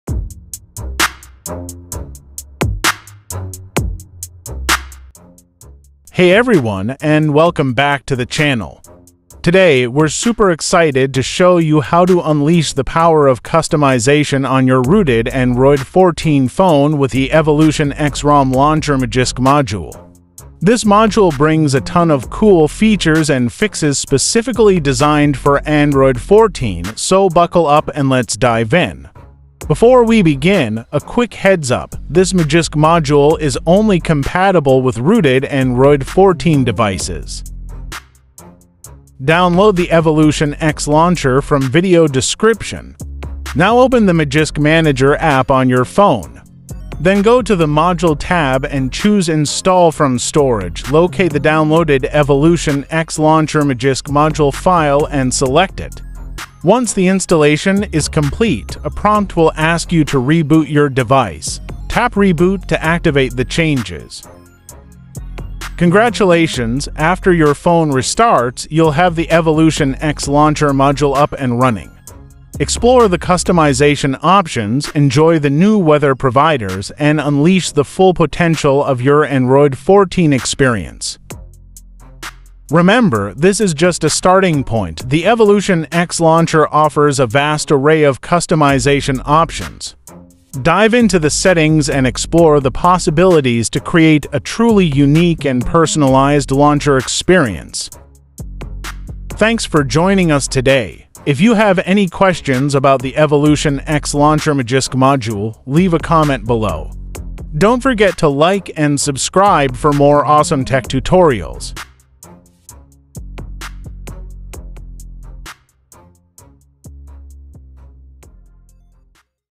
Evolution X Launcher for Rooted Android 14 Installation Guide
Take your rooted Android 14 phone to the next level with the Evolution X Launcher Magisk module! This powerful tool unlocks a world of customization

Changelogs :
Fixed ThemePicker across ROMs
Easy access to OmniJaws customization tool
New Accu Weather & Weather Channel providers
Universal icon styles for any ROM